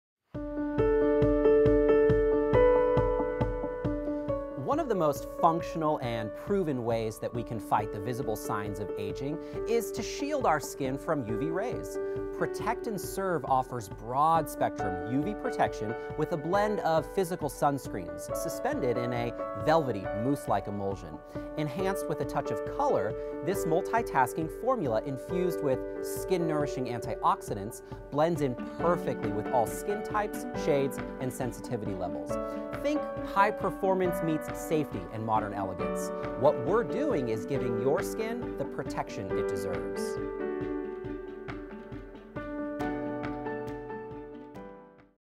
mybody PROTECT & SERVE Tinted Sun Shield SPF 30
A high-performance tinted physical sunscreen protects the skin with broad spectrum UVA//UVB SPF 30 and serves as an elegant make-up primer for a smooth, weightless finish.